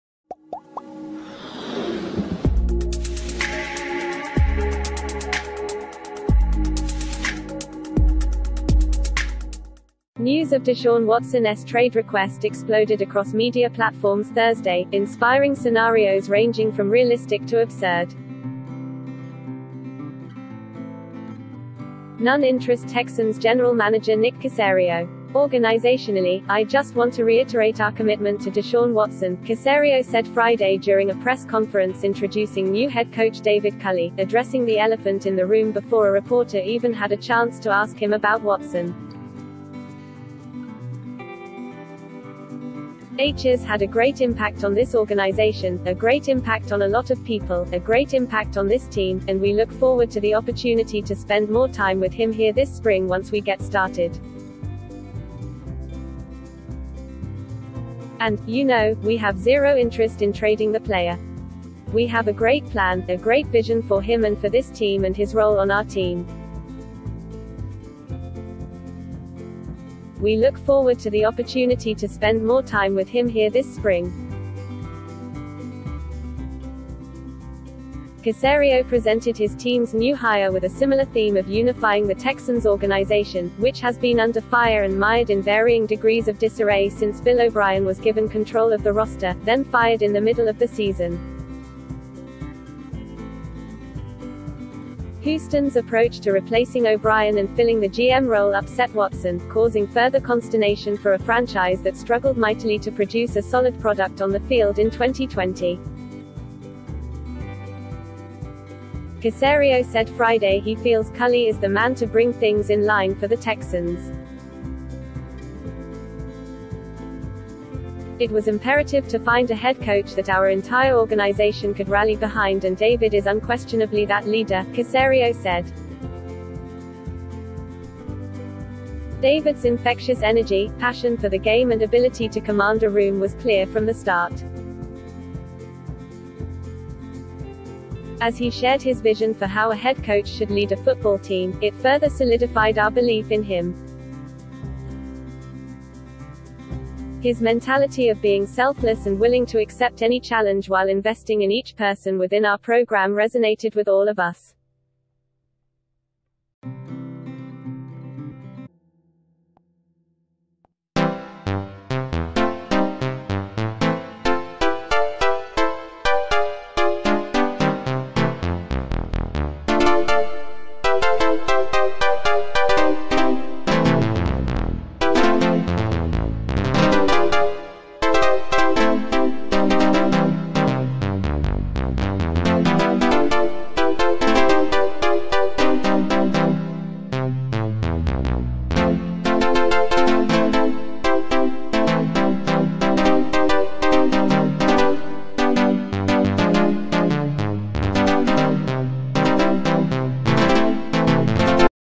At the onset of Friday's introductory press conference for David Culley, general manger Nick Caserio
✍  News of Deshaun Watson's trade request exploded across media platforms Thursday, inspiring scenarios ranging from realistic to absurd.    None interest Texans general manager Nick Caserio.    "Organizationally, I just want to reiterate our commitment to Deshaun Watson," Caserio said Friday during a press conference introducing new head coach David Culley, addressing the elephant in the room before a reporter even had a chance to ask him about Watson. "He's had a great impact on this organization, a great impact on a lot of people, a great impact on this team, and we look forward to the opportunity to spend more time with him here this spring once we get started.    "And, you know, we have zero interest in trading the player. We have a great plan, a great vision for him and for this team and his role on our team. We look forward to the opportunity to spend more time with him here this spring."    Caserio presented his team's new hire with a similar theme of unifying the Texans organi ...